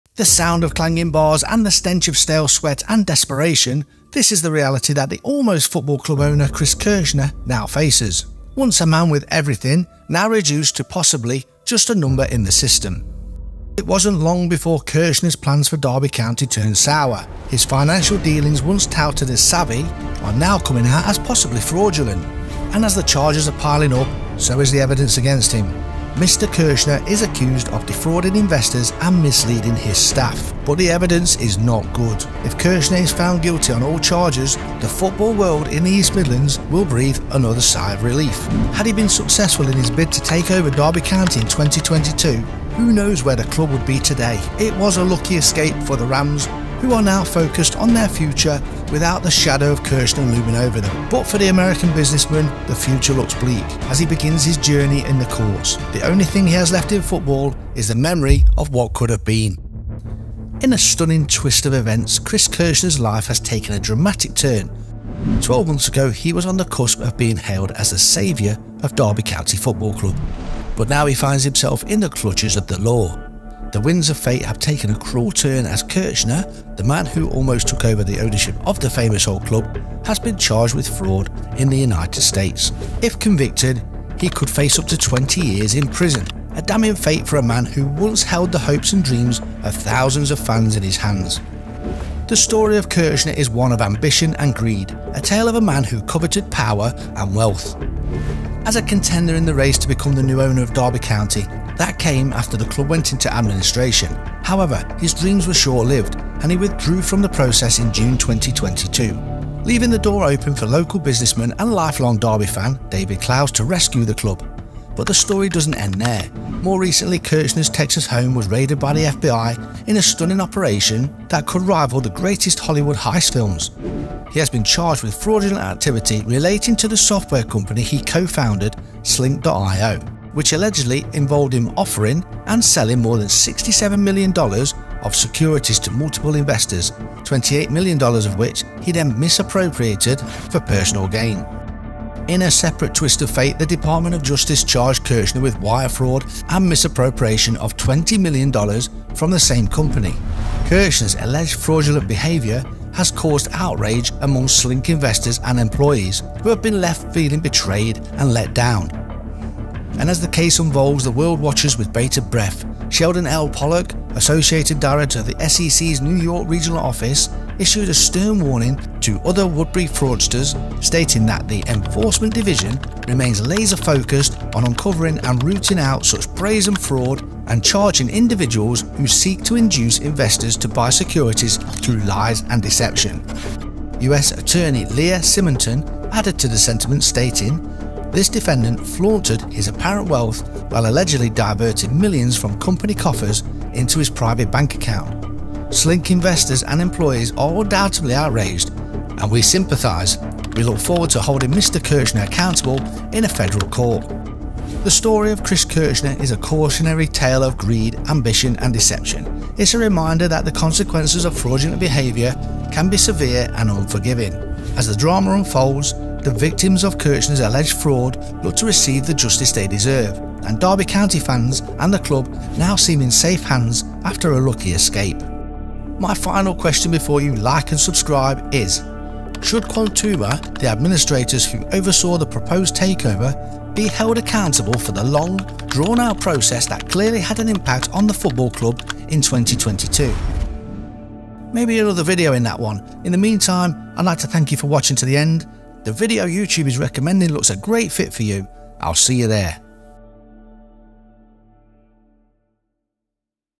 Derby County's Darkest Hour: How Chris Kirchner's Fraud Arrest Nearly Cost the Club Everything
Are you ready to uncover the shocking truth about the man who almost took over Derby County? In this video, we delve into the story of Chris Kirshner, the businessman whose ambitions were brought crashing down by allegations of fraud.

The excitement and anticipation that surrounded Kirshner's proposed takeover of Derby County Football Club. Fans were eagerly anticipating a new era of success, with Kirshner promising big investments and a fresh approach.

But as the details of Kirshner's business dealings began to emerge, doubts started to creep in. Soon, allegations of fraud and financial irregularities were swirling around the would-be owner, casting a shadow over his bid to take over the club.

In this video, we explore the evidence against Kirshner, examining the complex web of companies and transactions that raised red flags with regulators. We also hear from experts in the world of finance and law, who shed light on the legal and ethical implications of the case.

And we don't forget about the impact on Derby County. What would have happened if Kirshner's takeover had gone through? How close did the club come to being embroiled in a major financial scandal? We take a deep dive into the implications of the failed takeover, and what it means for the future of the club.

This video is a must-watch for football fans, business enthusiasts, and anyone interested in the world of finance and regulation. Join us as we uncover the shocking story of Chris Kirshner's arrest and the lucky escape of Derby County Football Club.